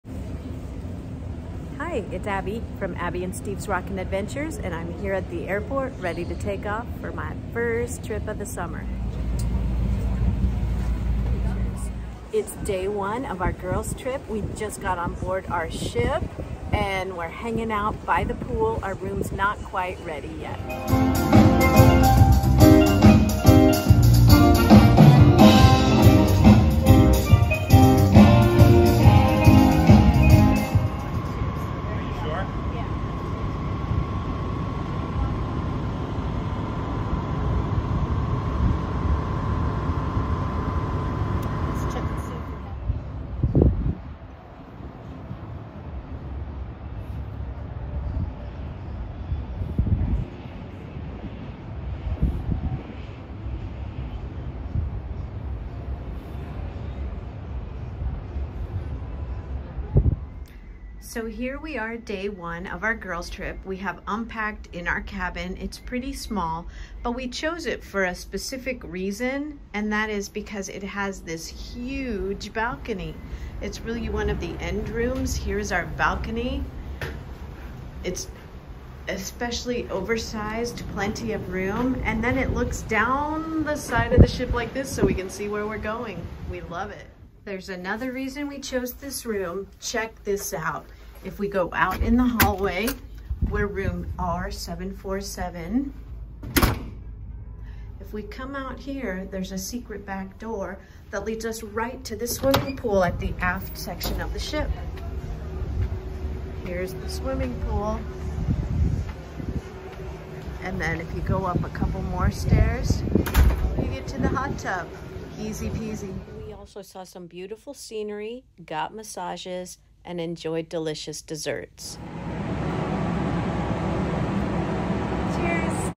California Cruising on the Emerald Princess day 1@abbeystevesrockinadventure7893
Day 1 of our Girls Trip 2023; hanging out on the ship.